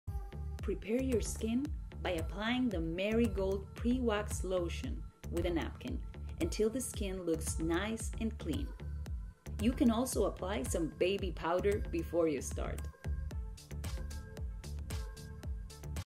Step 6/10 on How to use waxup roll on wax at home
Many people ask us how to prep skin for waxing?
Before you begin waxing, you should first apply a pre wax cleanser like waxup Pre Wax Lotion Step 1 to help remove any impurities and residues left from oils or creams you may have used on your skin.
Don't forget to dry lotion residues with a paper towel, to make sure the skin is completely dry before starting waxing. You can also apply some baby powder before you start.
This will help the wax stick to the hair that will be removed in your waxing session.

We know you want to start using your roller waxing kit on the skin, we are getting closer to start removing all your unwanted hairs. Now go to Step 7.